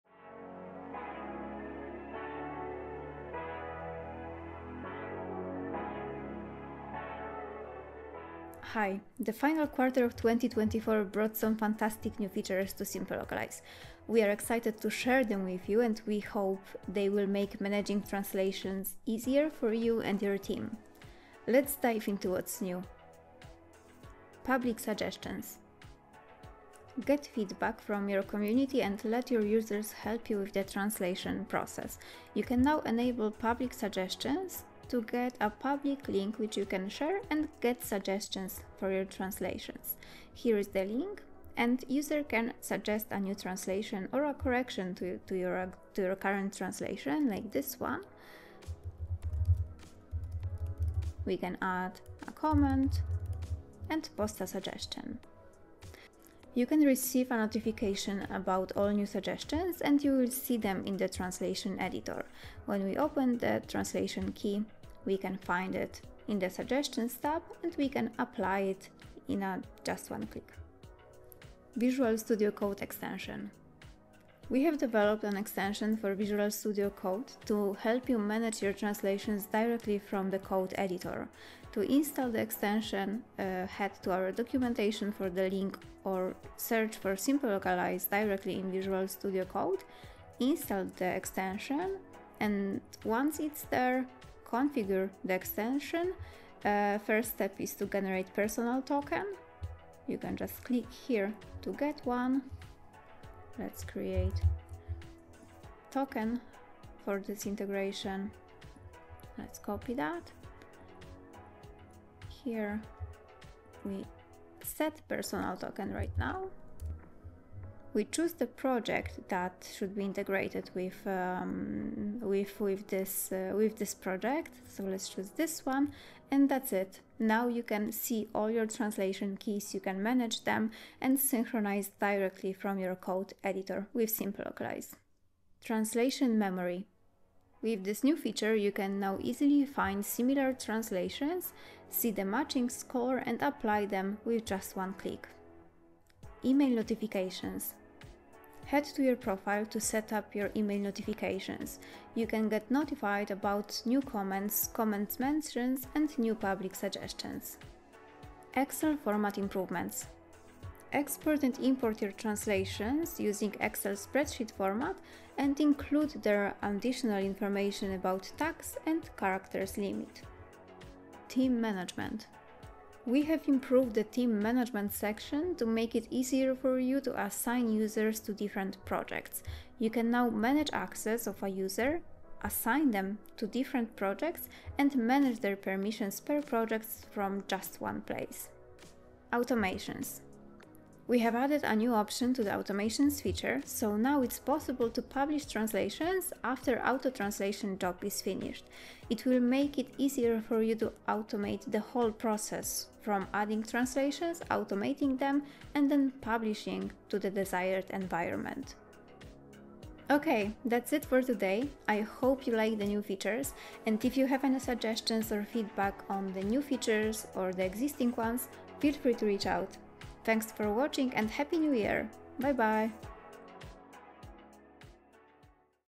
What's new in SimpleLocalize - Q4 2024
Welcome to our Q4 2024 update! Join us as we dive into the latest developments, milestones, and exciting news from the last quarter of the year.

0:00 Intro
0:24 Public suggestions for translations
1:12 Visual Studio Code extension
2:16 Translation memory
2:29 Email notifications
2:39 Excel format improvements
2:52 Team management
3:10 Publishing via Automations
3:29 Outro